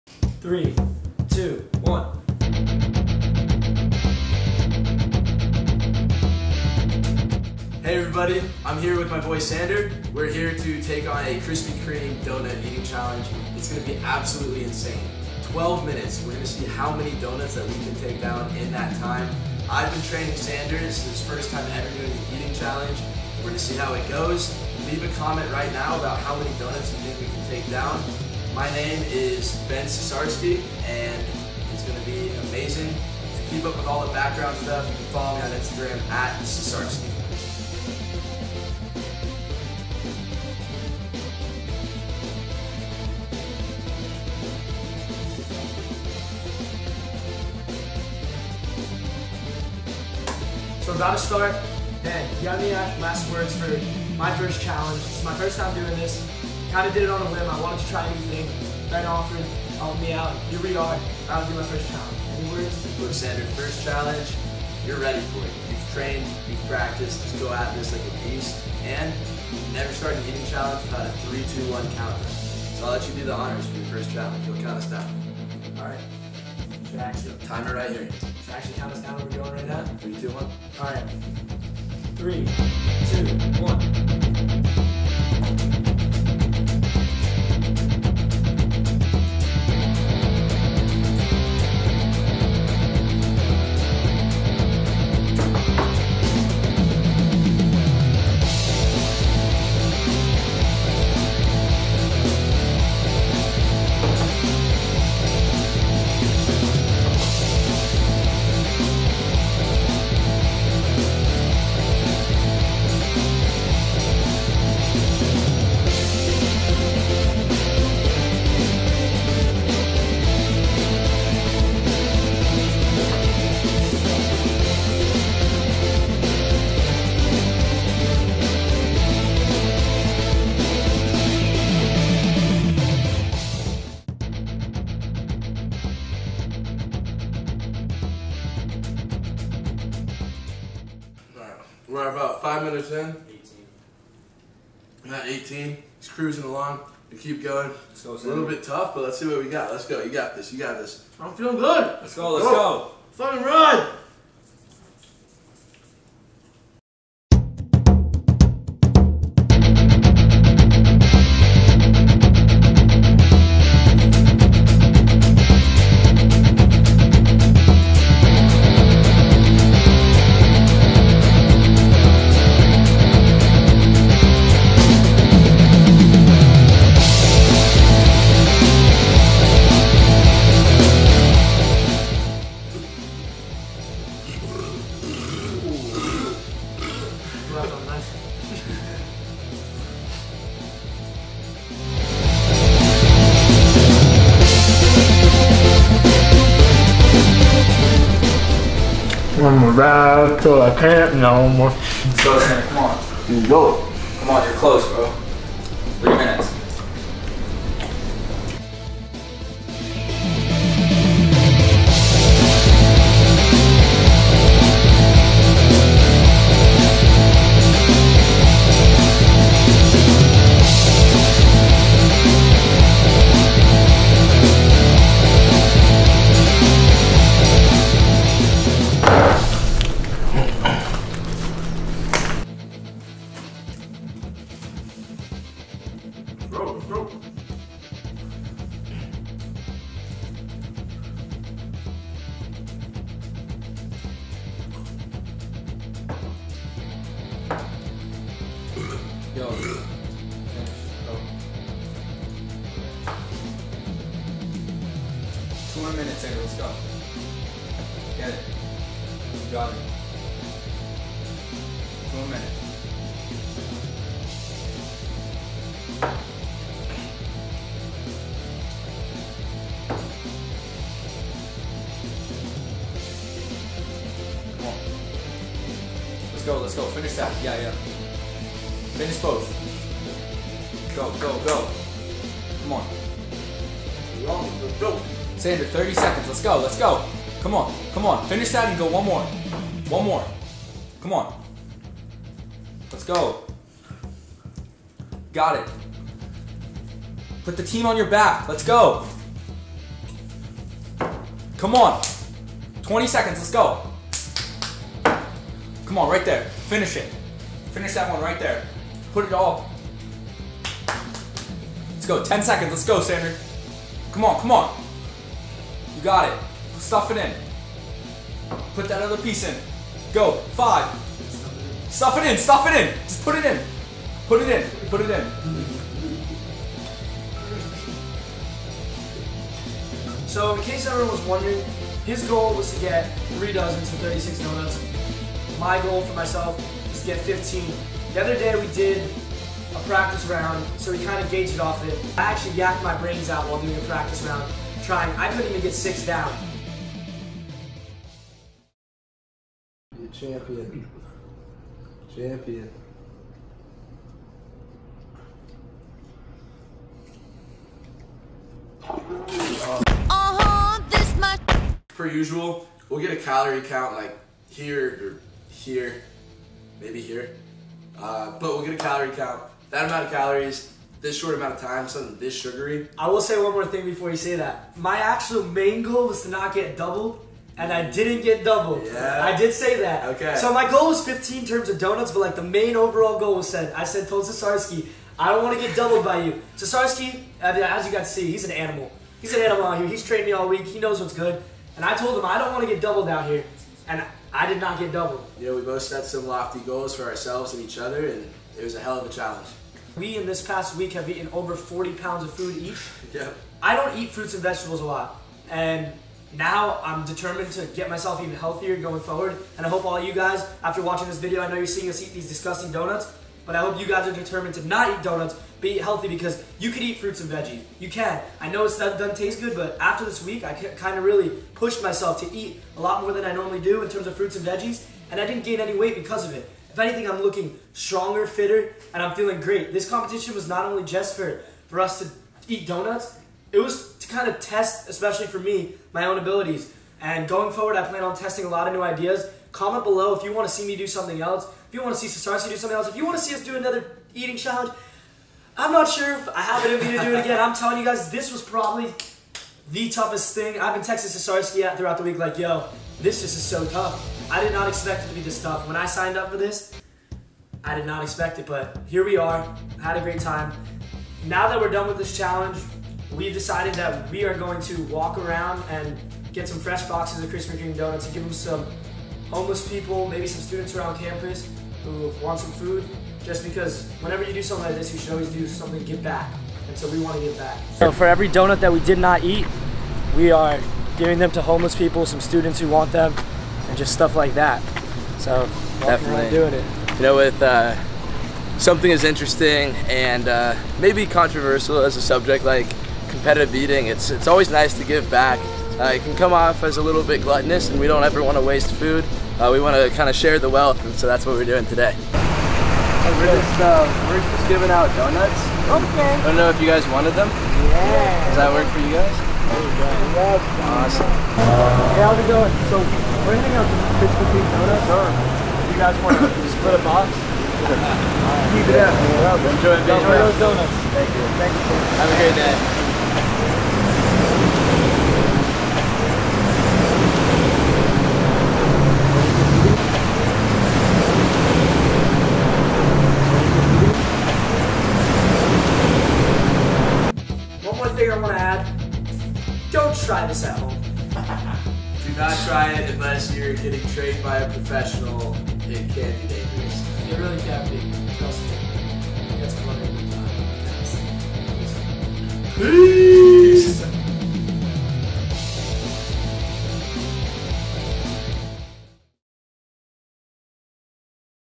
Krispy Kreme Donut Eating Challenge! (8,000 Calories)
In this video I attempt to eat as many Krispy Kreme donuts as I can in 12 minutes with my friend and TLC Celebrity Sander Jennings. Check it out to see us take down nearly 8,000 calories of donuts!